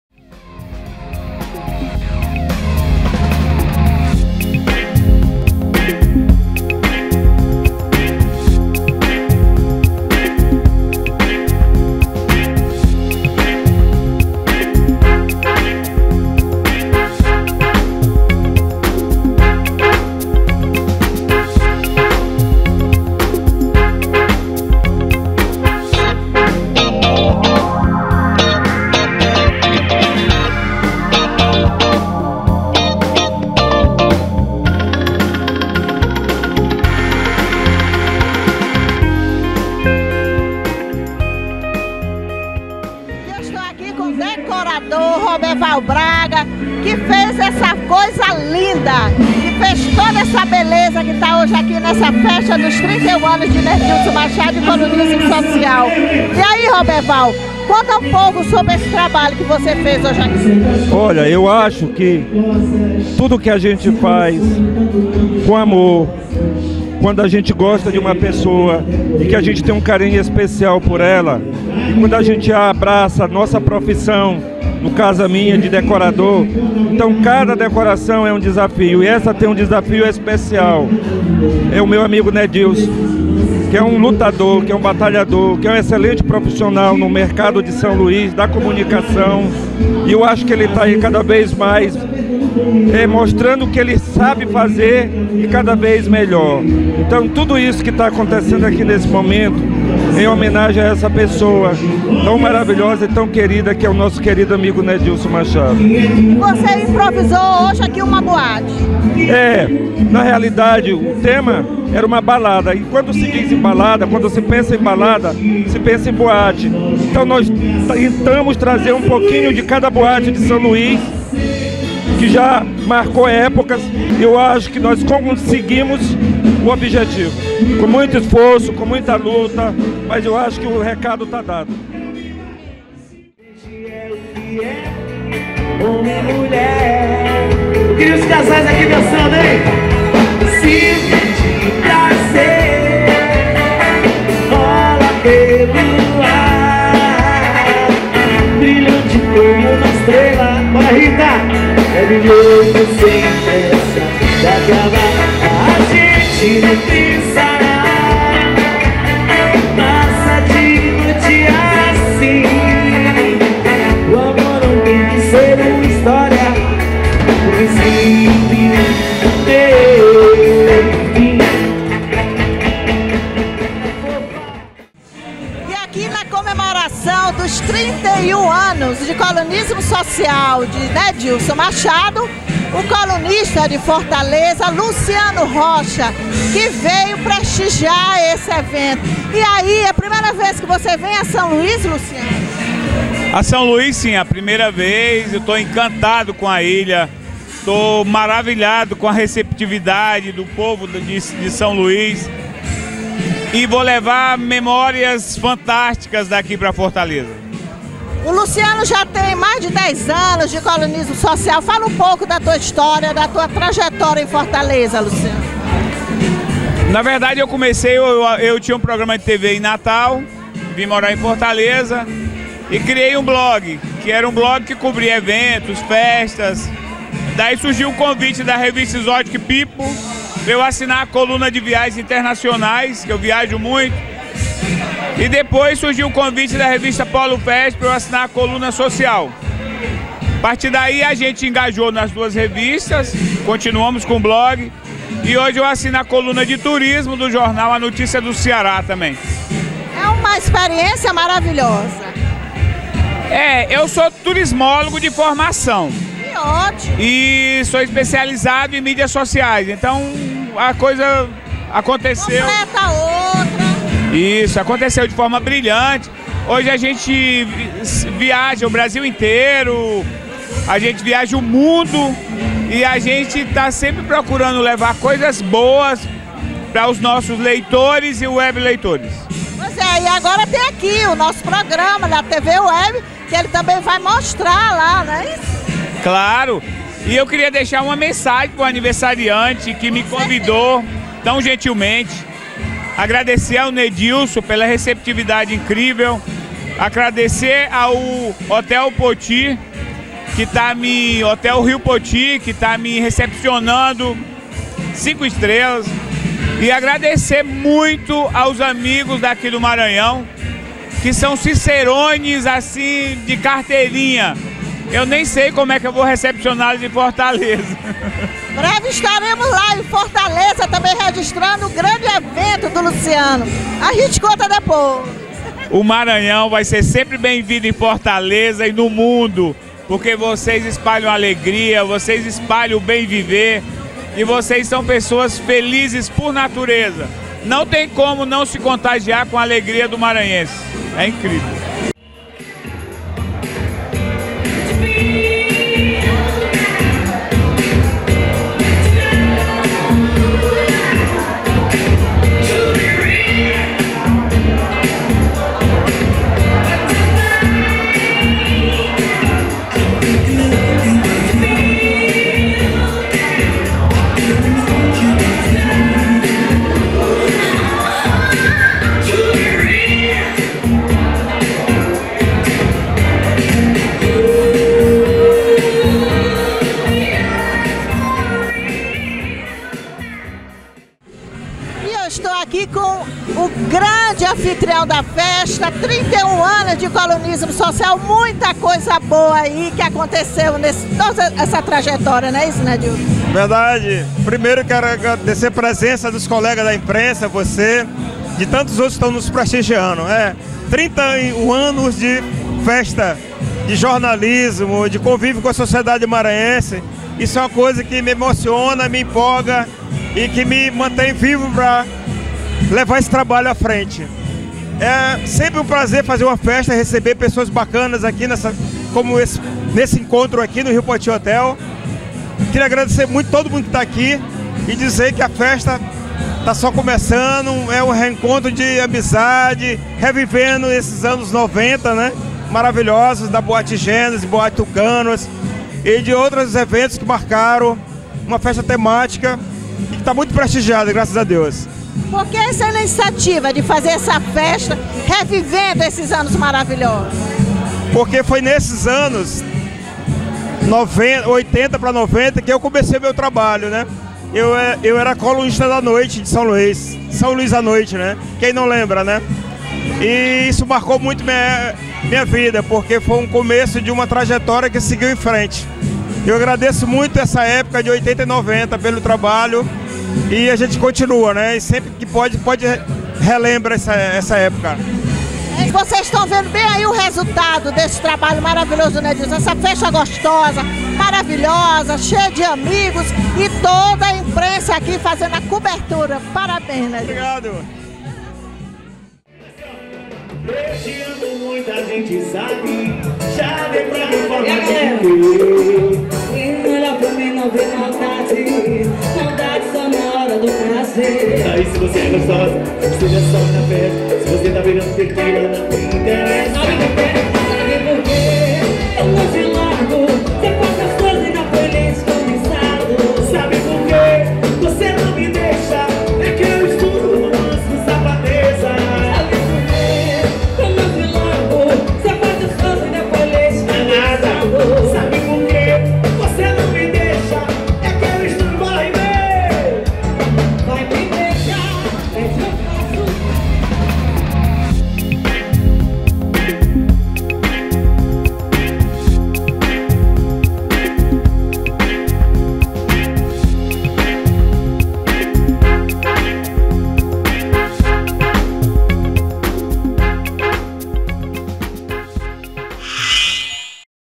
A Festa de 31 anos de Jornalismo de NEDILSON MACHADO
Produção: TV ARTESANAL
Apresentação: Orquídea Santos
Edição: Mhario Lincoln
Imagens: Adelman Baltazar
São Luís, SET.2018